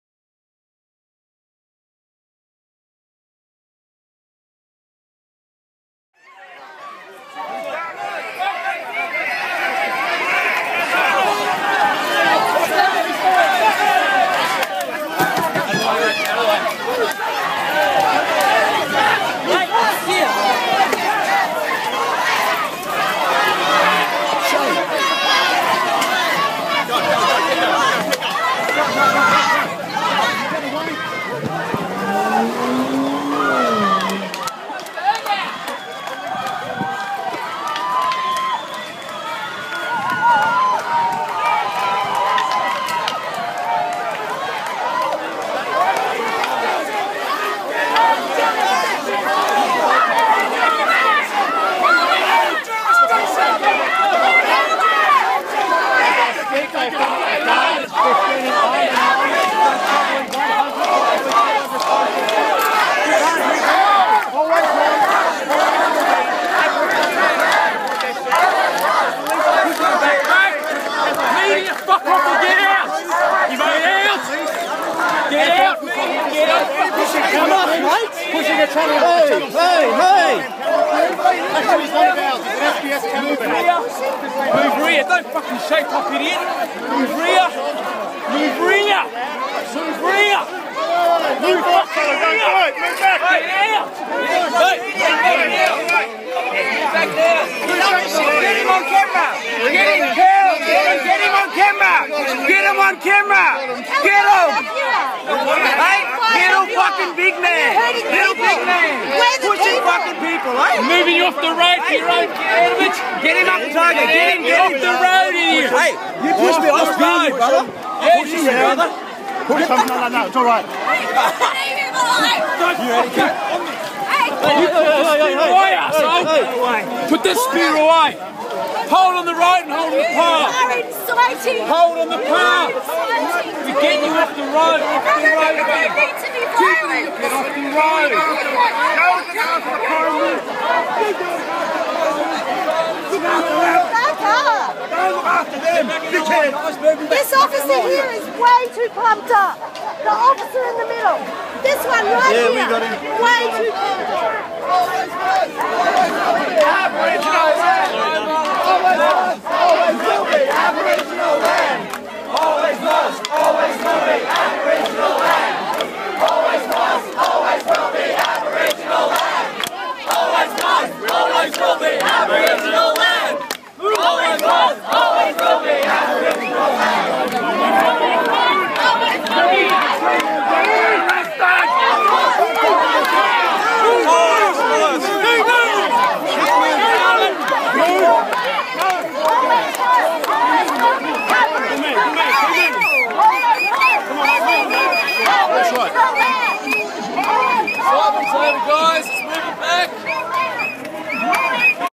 Tent Embassy protestors and PM's security assaulted by riot police
Tent Embassy protestors assaulted by riot cops